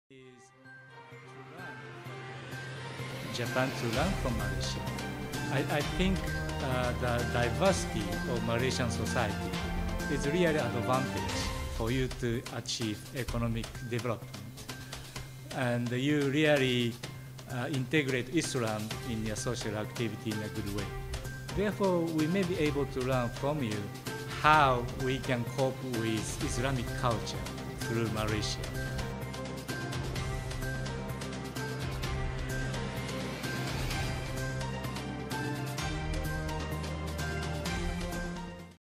Jepun berminat terokai keunikan budaya Malaysia dan industri halal - Duta Jepun
Menjangkau usia 40 tahun Dasar Pandang Ke Timur, Jepun dan Malaysia terus aktif meneroka potensi kerjasama dalam bidang baharu.

Terbaharu, Duta Jepun ke Malaysia, Takahashi Katsuhiko memaklumkan, Jepun berminat mempelajari keunikan budaya Malaysia yang menjadi asas kepada kemajuan sosioekonomi.

Malah berminat meneroka potensi industri halal, selain kewangan Islam. Menerusi Dasar Pandang Ke Timur, seramai 26 ribu rakyat Malaysia telah menerima latihan dan pendidikan di negara matahari terbit itu sejak diperkenal pada 1982.

Jepun kini aktif menawarkan kepakaran kepada Malaysia dalam aspek pengurusan risiko bencana dan isu negara tua. Katsuhiko menjadi tetamu wawancara khas stesen radio TraxxFM di Angkasapuri.